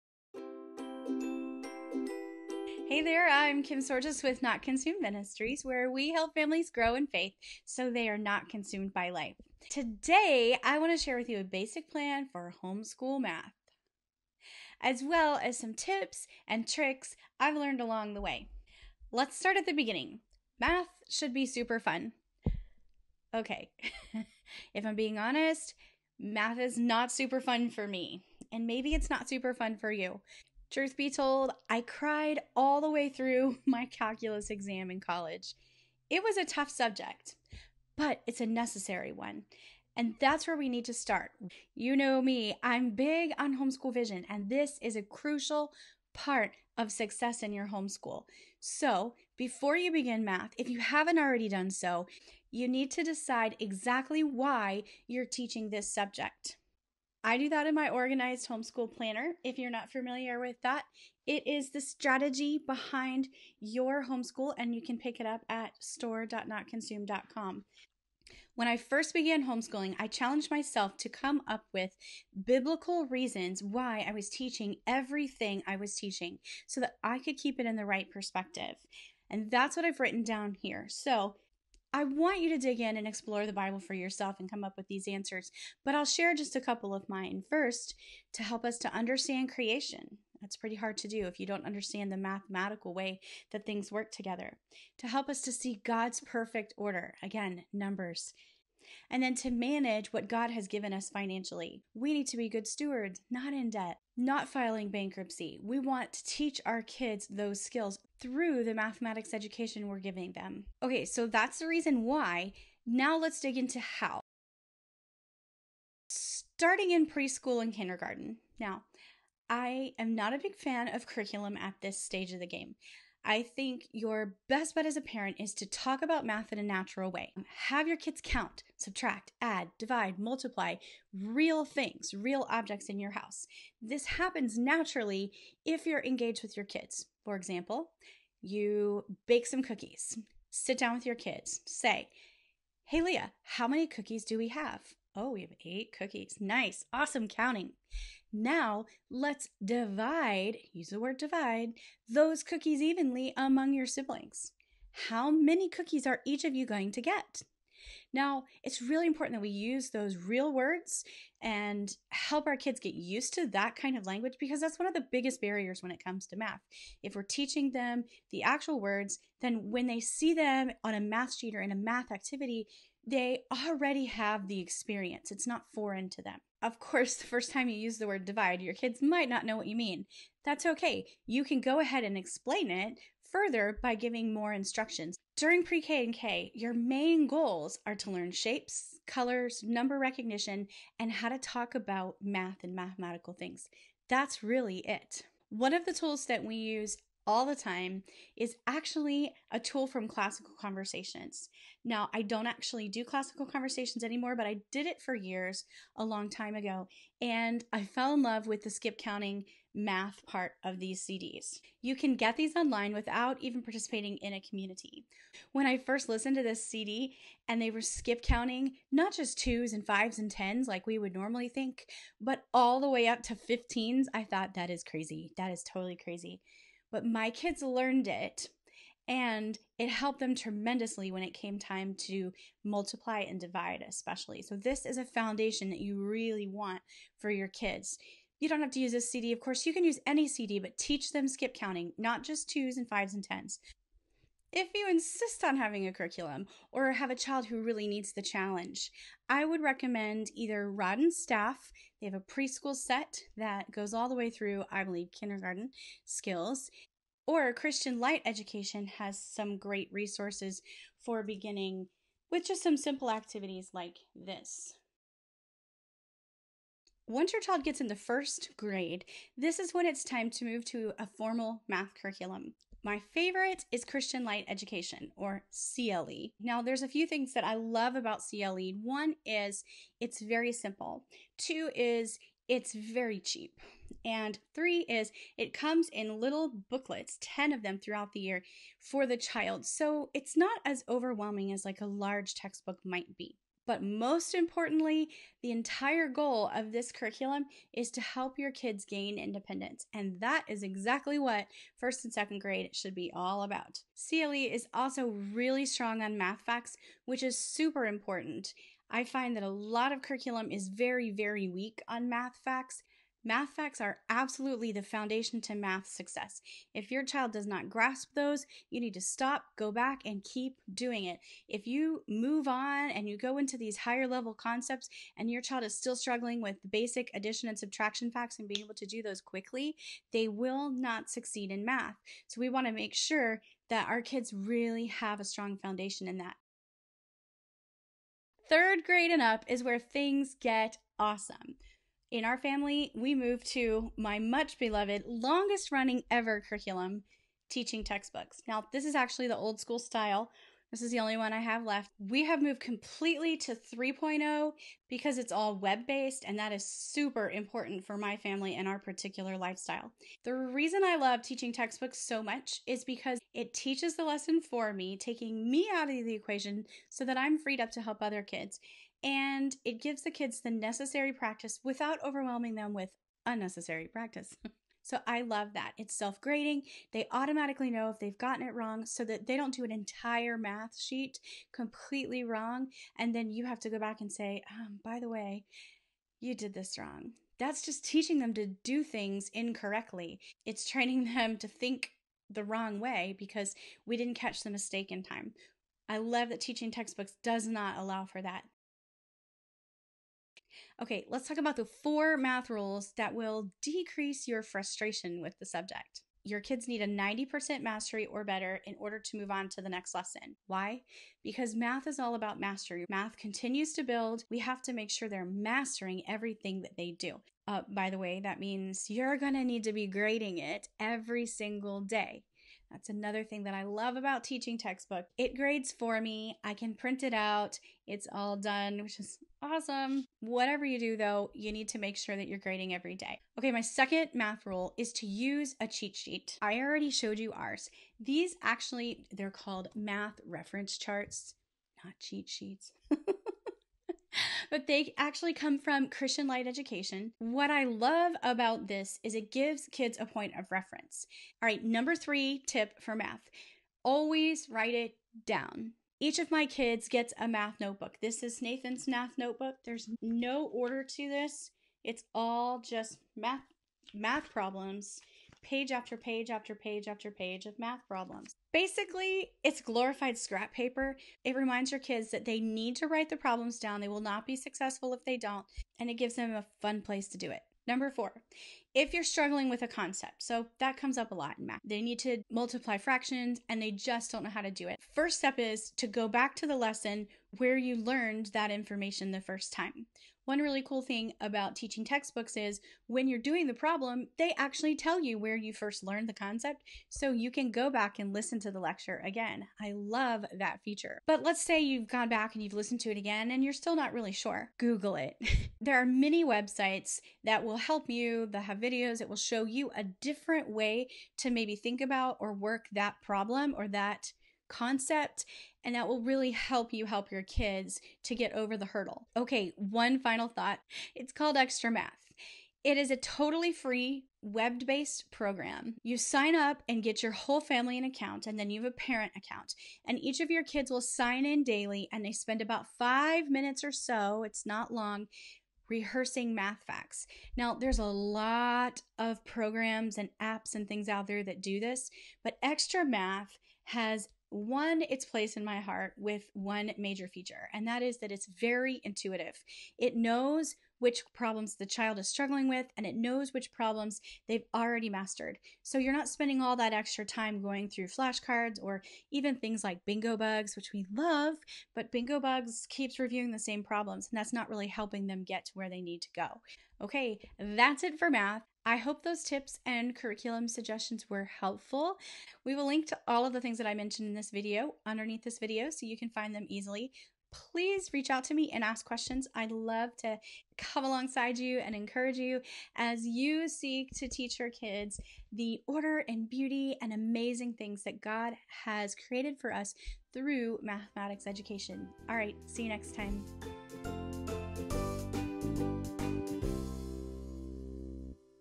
Homeschool Math: Tips, Tricks and Curriculum Ideas
Head over to our new YouTube channel where you'll find practical tips for Bible study, homeschooling, and family life!

Math. Oh, Math. Regardless of how we feel, it’s a necessary subject, so let’s conquer it with confidence as we remember that the study of numbers points to the order and majesty of God. I’ll walk you through my strategy for teaching math from preschool to high school, including tips and curriculum suggestions.

Find the links to the resources I mentioned and videos for all of the core subjects